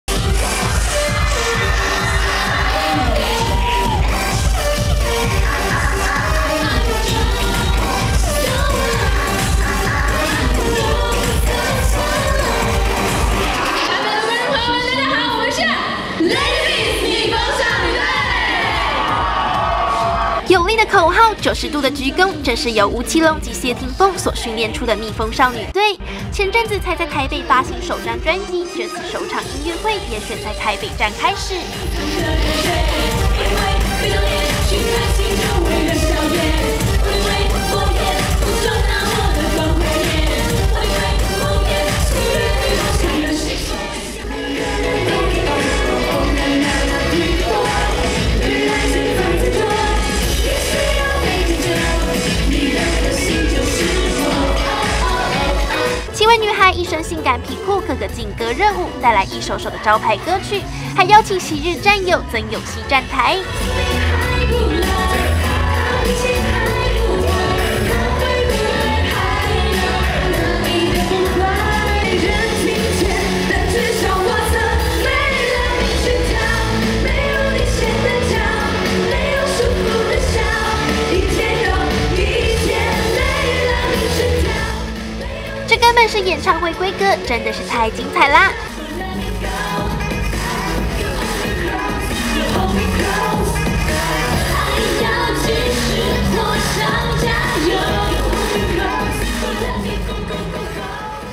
【3年前】蜜蜂少女隊台北站首唱 曬長腿露蠻腰性感爆棚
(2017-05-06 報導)

這是由吳奇隆及謝霆鋒，所訓練出的蜜蜂少女隊，前陣子才在台北發行首張專輯，這次首場音樂會，也選在台北站開始。